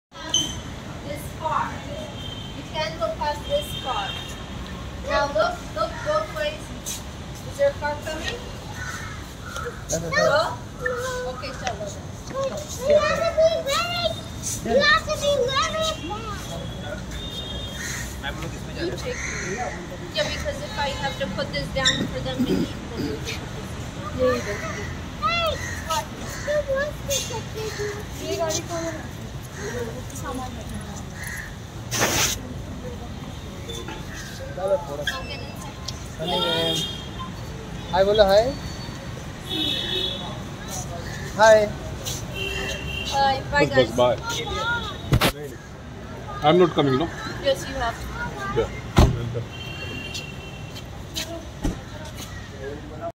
Glamorous Sunny Leone With Kids Nisha Kaur Weber| Asher Singh Weber| Noah Singh Weber Snapped At Lax
Glamorous Sunny Leone With Kids Nisha Kaur Weber| Asher Singh Weber| Noah Singh Weber Snapped At Laxmi Industrial State In Andheri West Mumbai...